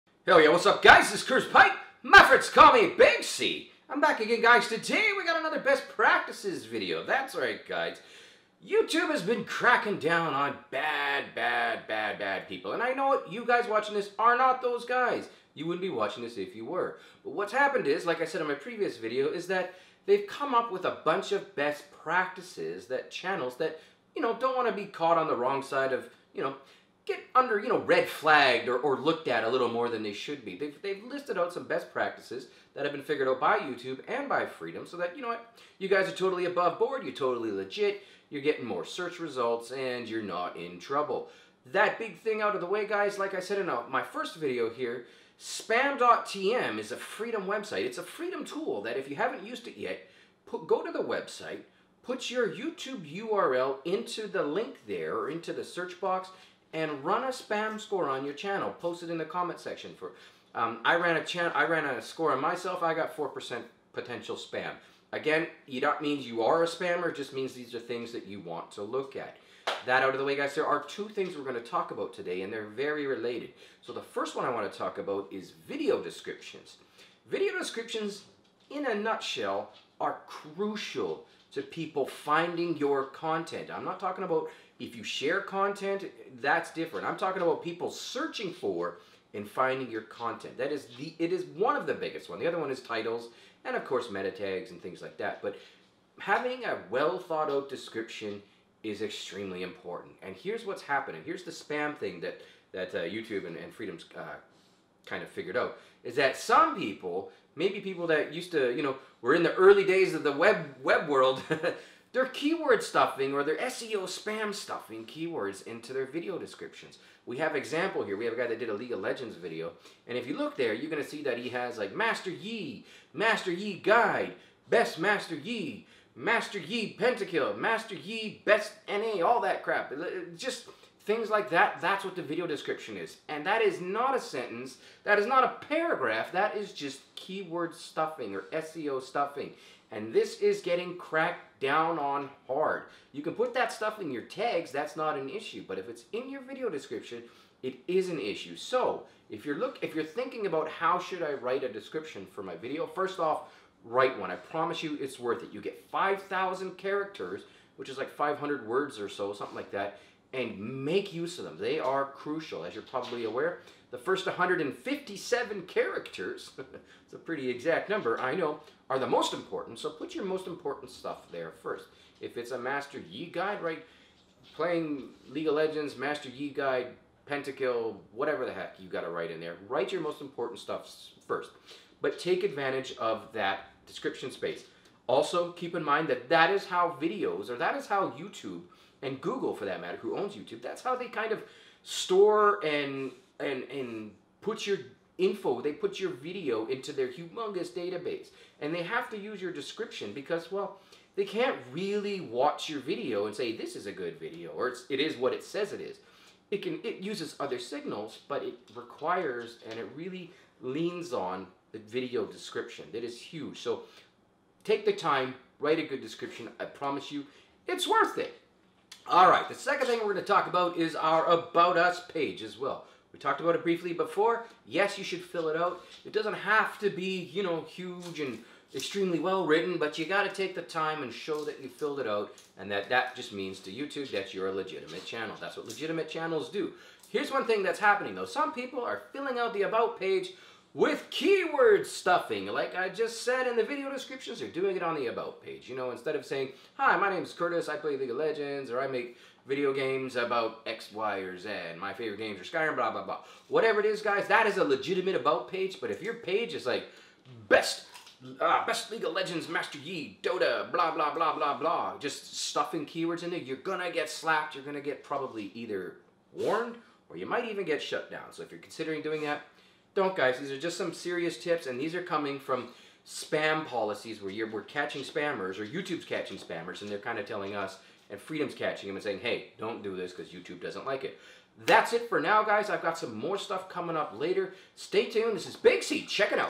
Youtube Best Practices - Video Descriptions and Keyword Stuffing - Careful!
Writing a good video description is one of the best things you can do to get more views on your videos.  That being said, stuffing keywords and search heavy terms can result in warnings / bans.  Watch the video to see some examples.

▼ Links

▼ Heartbeat

▼ Freedom!

▼ Music